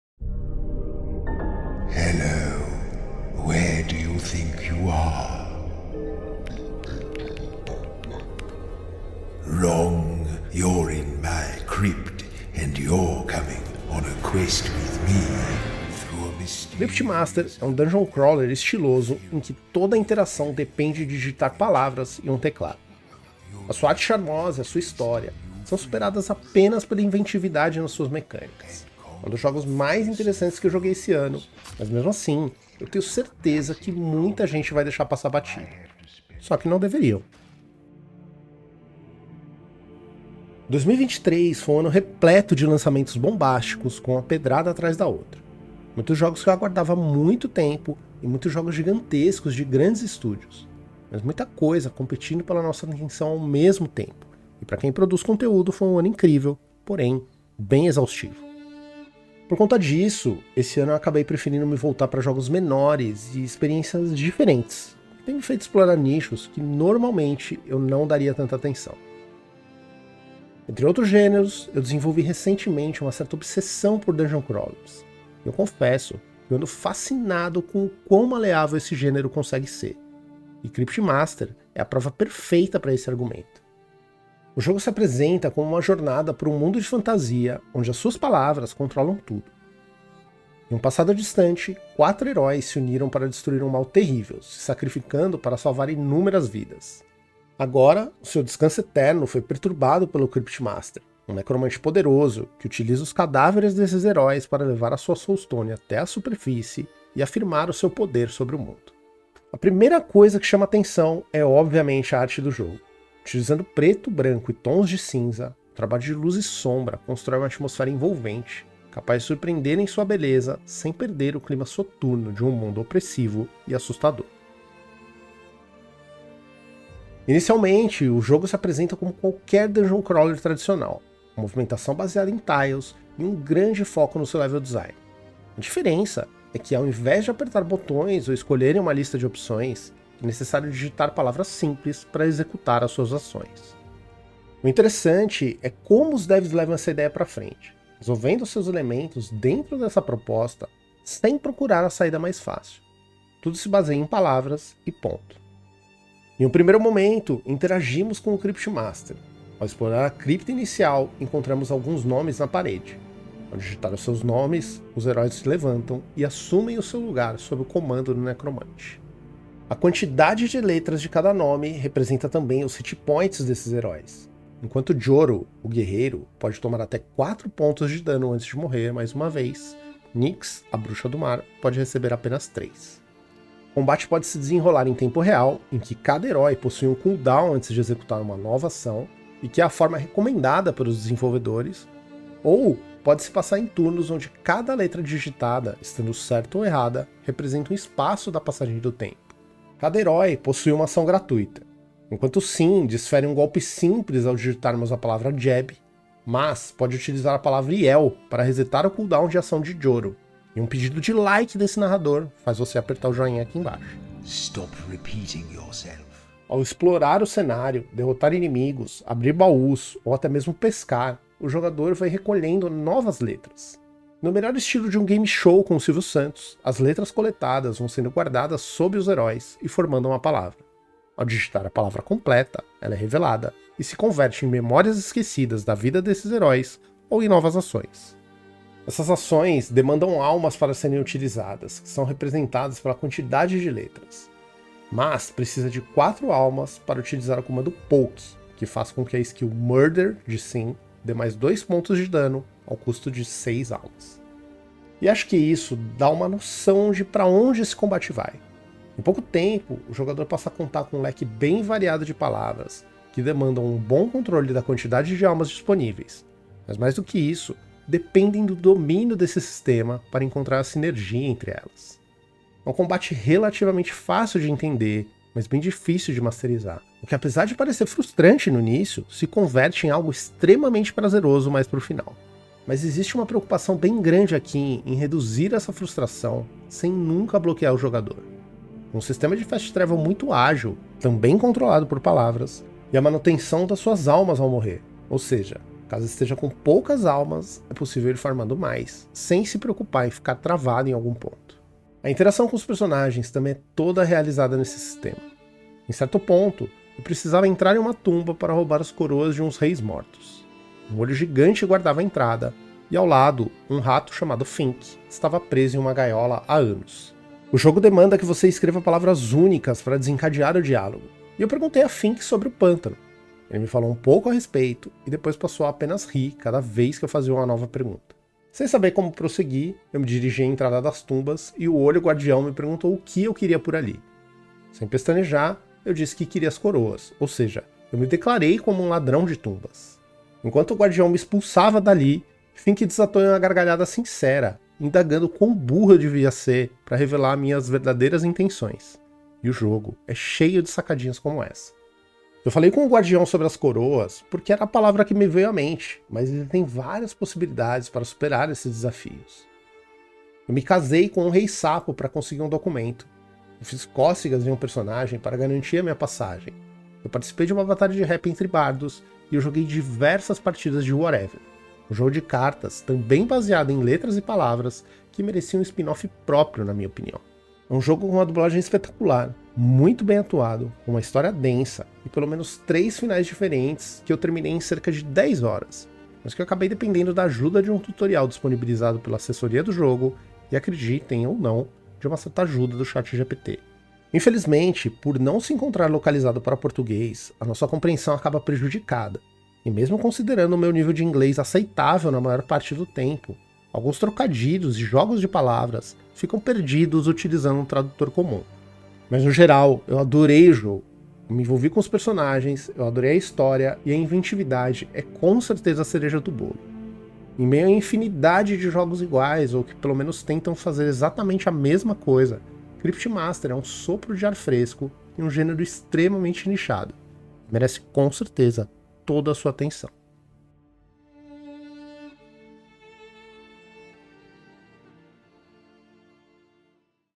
CANSOU dos VIDEOGAMES? CRYPTMASTER pode ser a SOLUÇÃO
Falando de videogame de um jeito diferente.

CRYPTMASTER é um dungeon crawler (explorador de masmorras) que te coloca na pele de 4 heróis e precisa sair de onde está apenas utilizando as palavras... mas as corretas!

Interaja com o mundo e conclua missões digitando ou falando as palavras que vierem à sua mente; cada encontro tem várias soluções a serem descobertas!

Viaje por reinos subterrâneos repletos de mistério e encontre portas falantes, sapos paqueradores, carniçais sarcásticos e muito mais.

As batalhas nas masmorras de CRYPTMASTER são alimentadas por palavras; decifre feitiços esquecidos e, em seguida, digite seus nomes em batalha para liberar o poder de seus heróis mortos-vivos.

Em CRYPTMASTER ocê encontrará constantemente novas mecânicas de jogabilidade, incluindo coleção de criaturas, combate com cartas, batalhas de rap entoadas por um bardo e muito mais.

Nos suporte, clicando no LIKE se gostou do vídeo❗️

👍Segue a gente no Twitter:
✔️Leandro - @OldPlayersND
✔️Thiago - @Danzzlopes